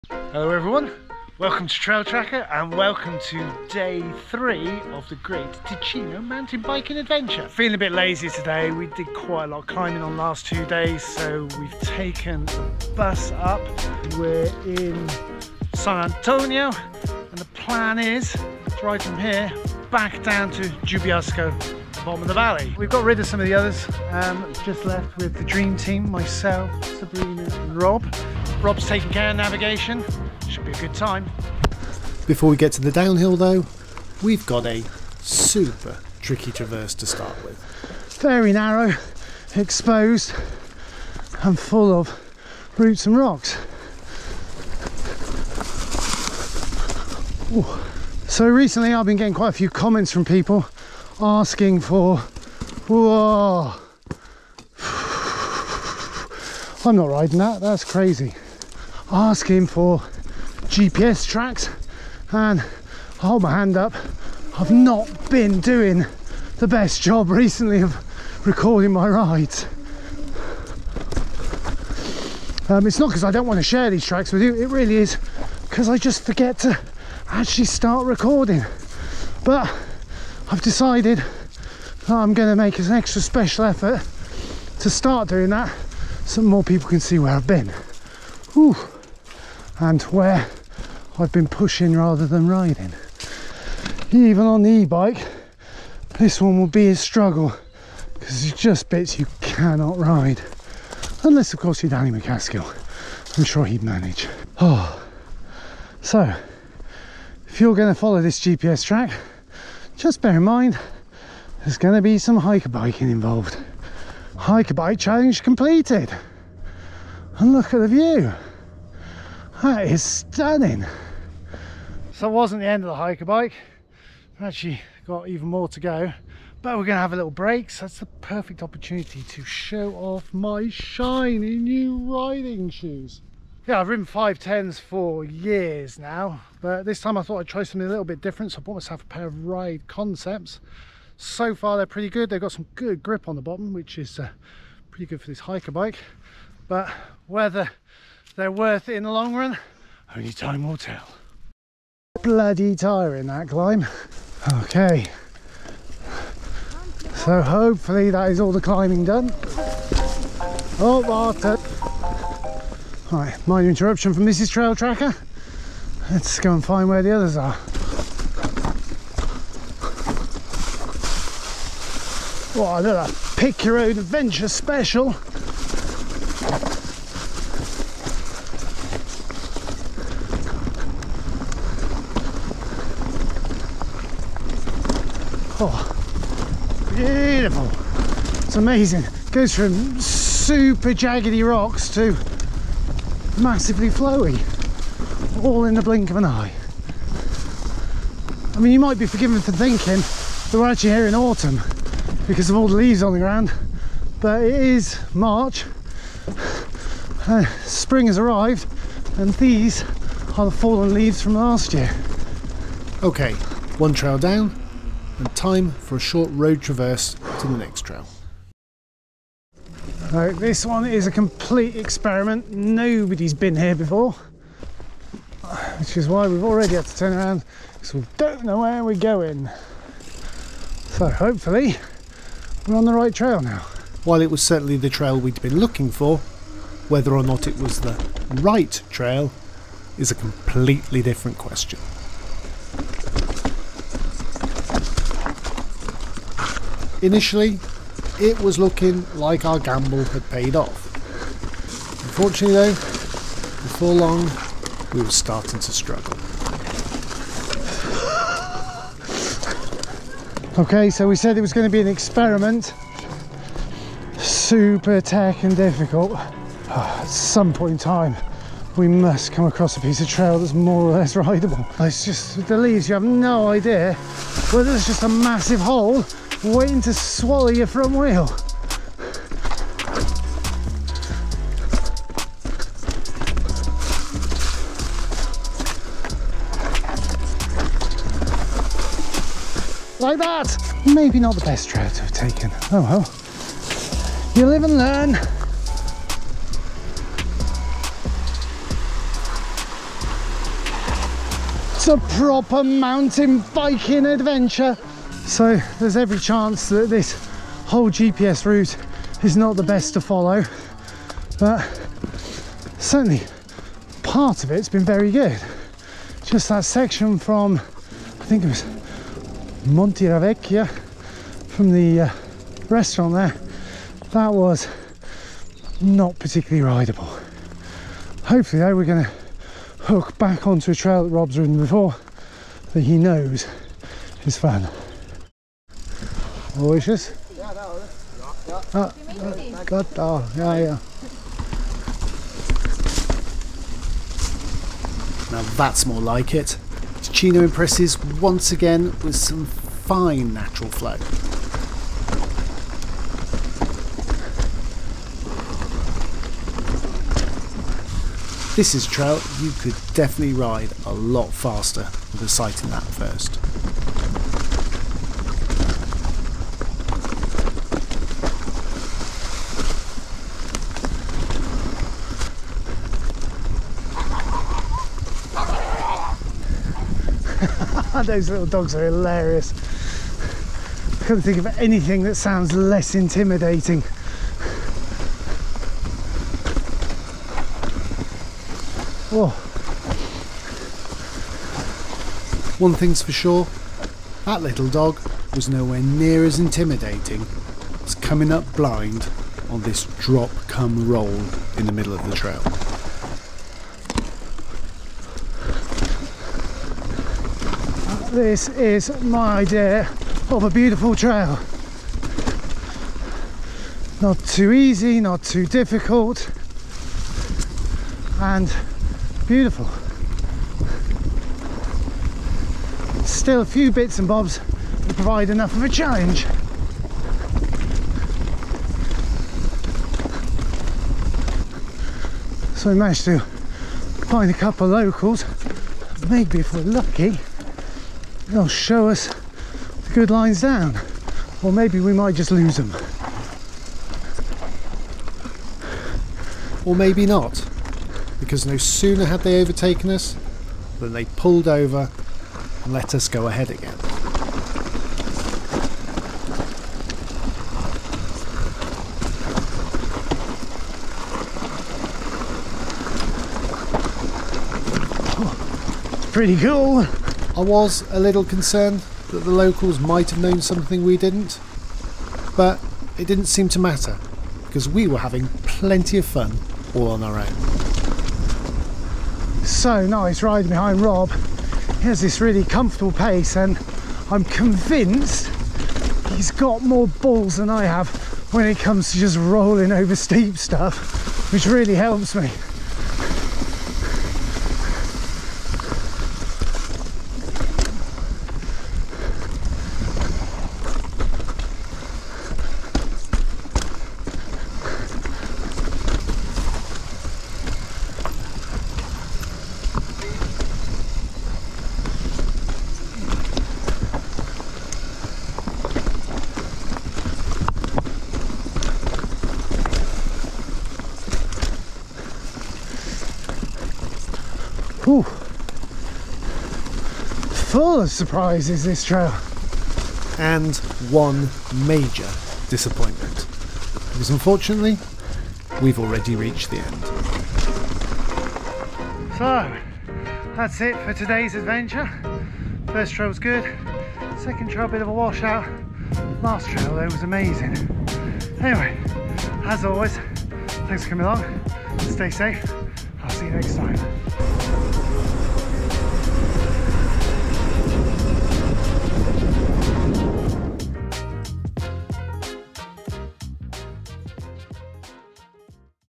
Ups and downs near Giubiasco (MTB Ticino)
Last of three days exploring in Ticino, and I think it's fair to that it didn't disappoint.